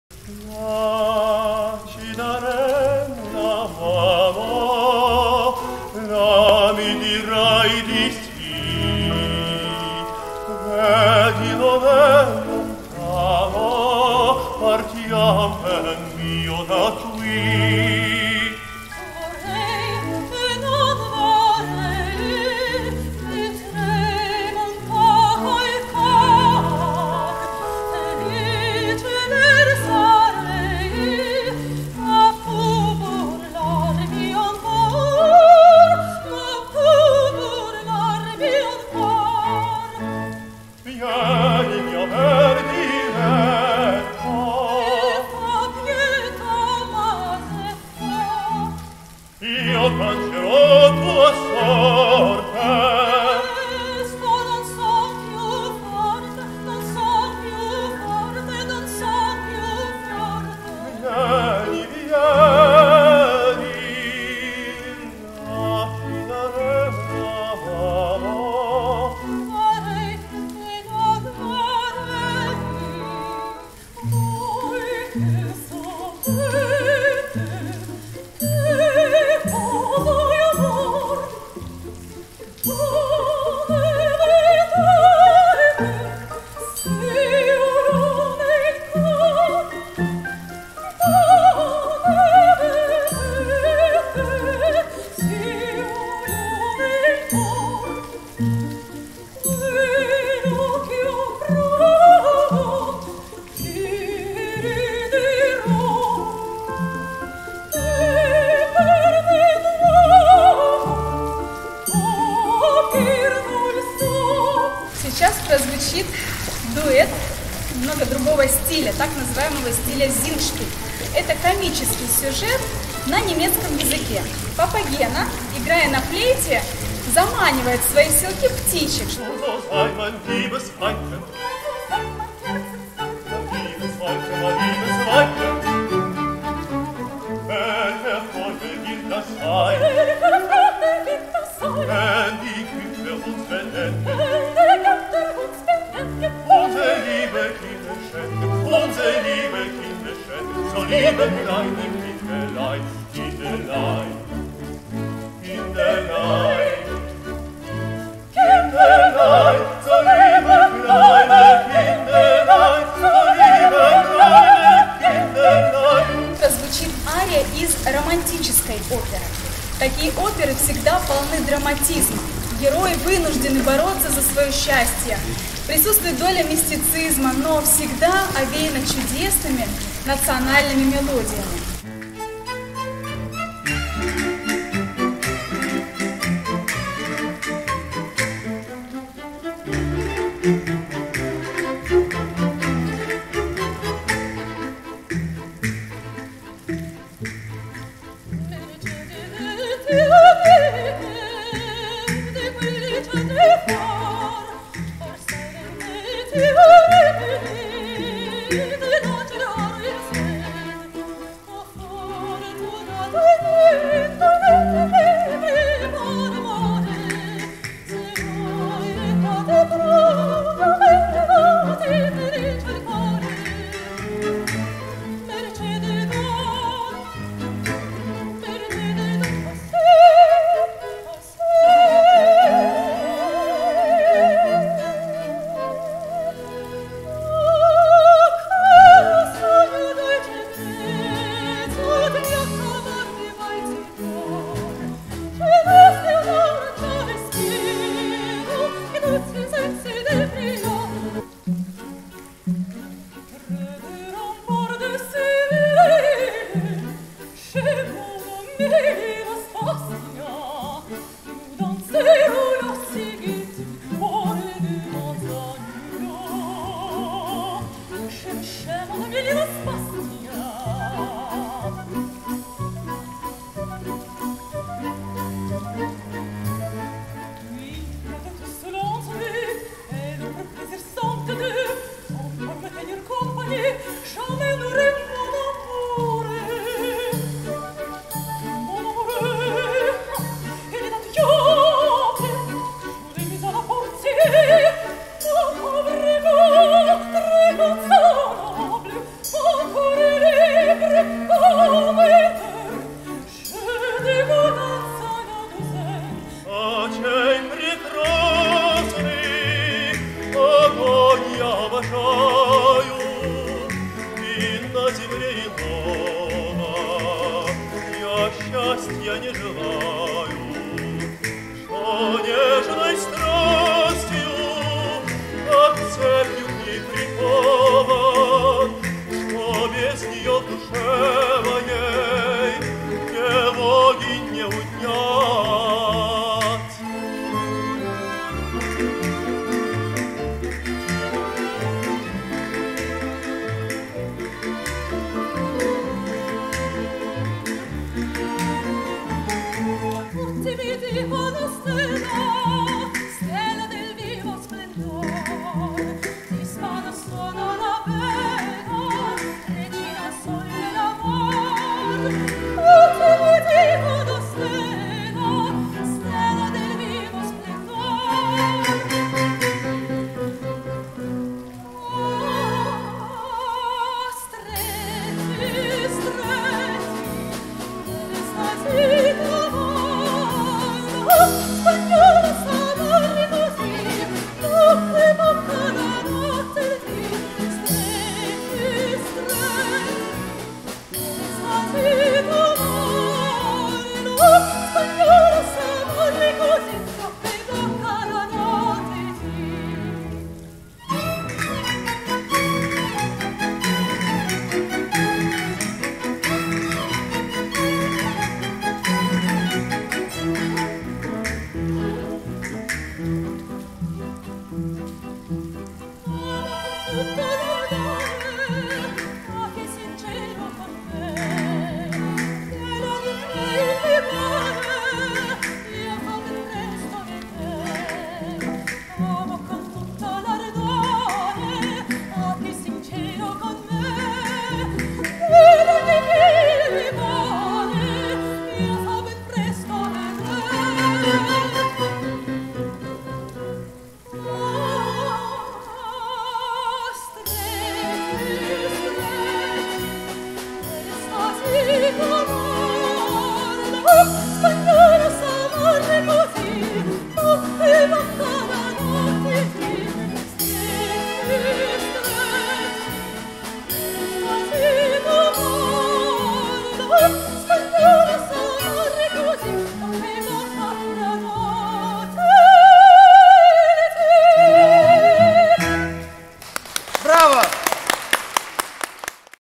"Музыкальный вернисаж", фрагмент # 02 (Ставропольская капелла "Кантабиле", 27.08.21)
Фрагмент программы "Музыкальный вернисаж" Ставропольской капеллы "Кантабиле" в т/к "Капитал" 27 августа 2021 г.
Художественный руководитель коллектива - Татьяна Кошанская. Солисты - Ильяс Батчаев, Елена Тонких, Кристина Дарчук.